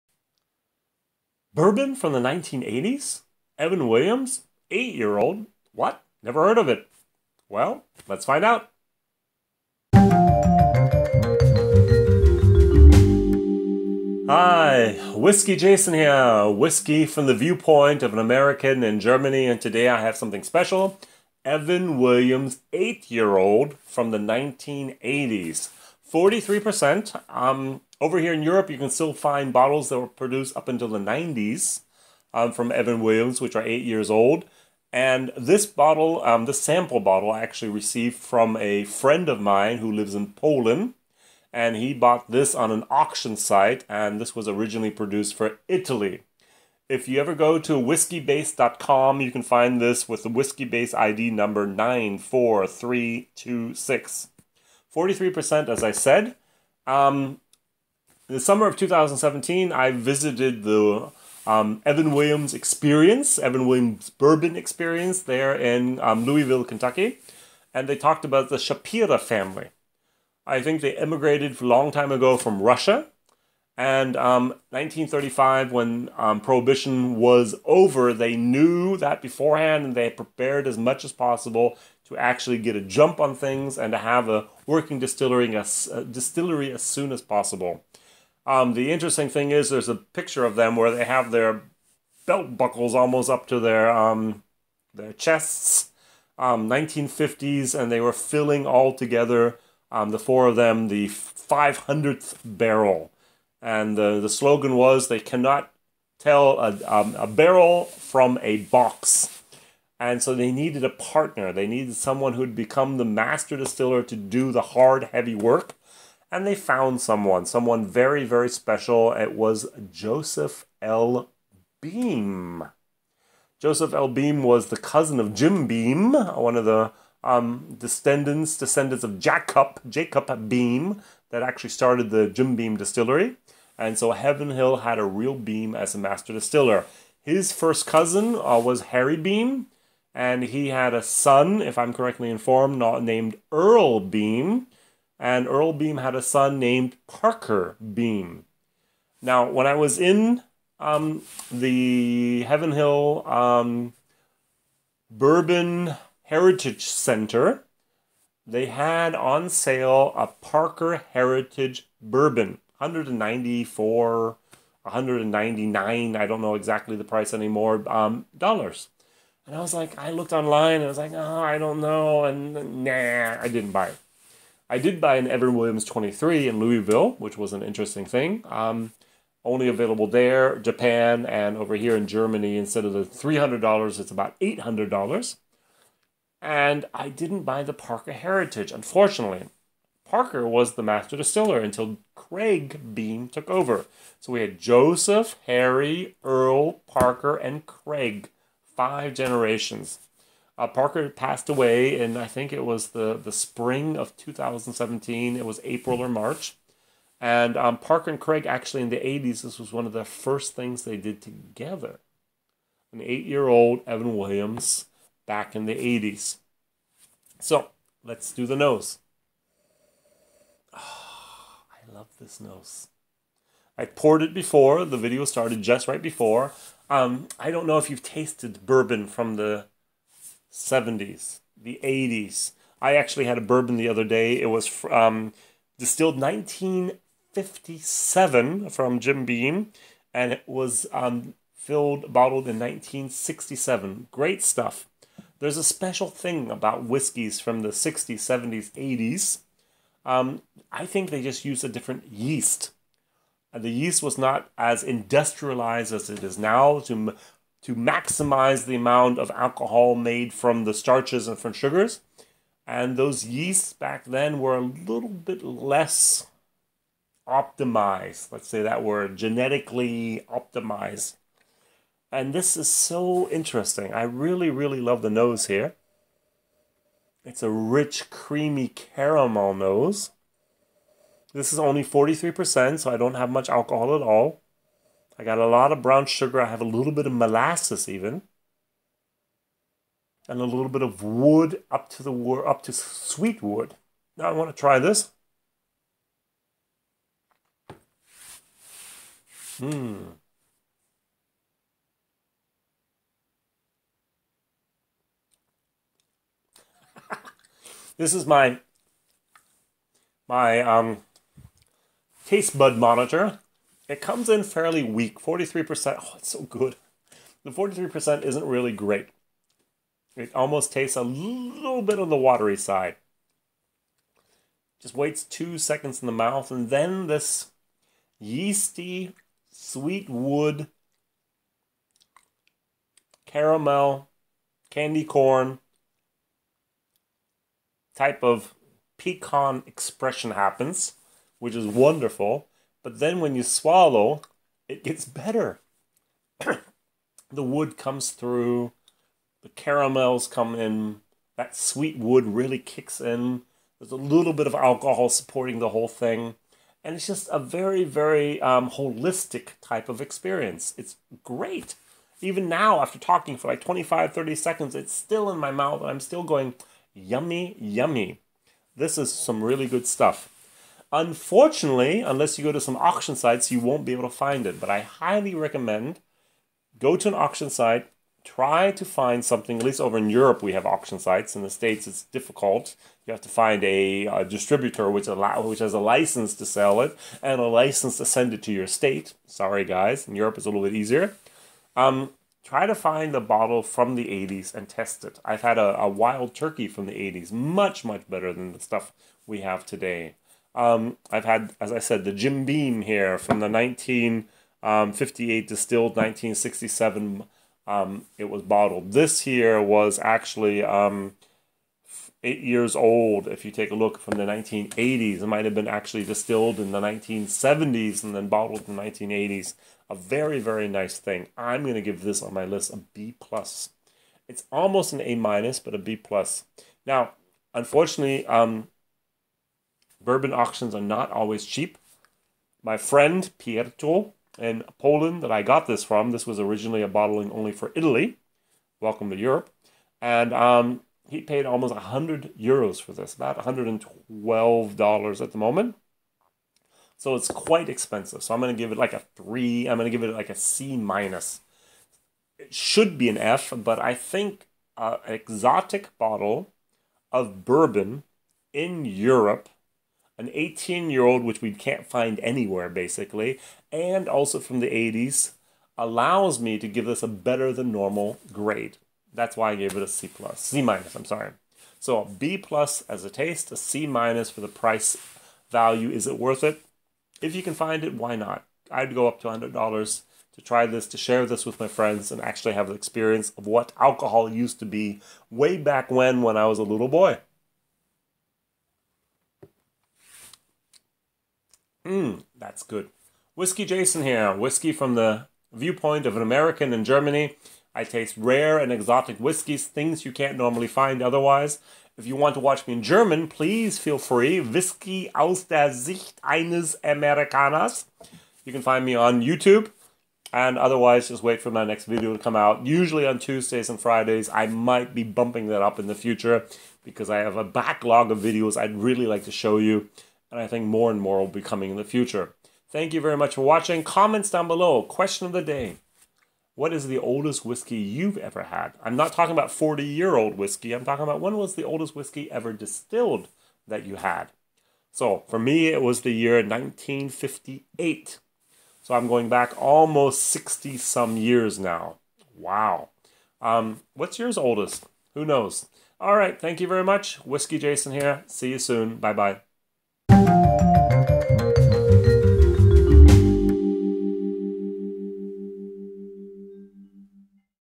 #49 - Evan Williams 8 Year Old Bourbon Whiskey review from WhiskyJason
WhiskyJason reviews Evan Williams 8 Year Old Bourbon Whiskey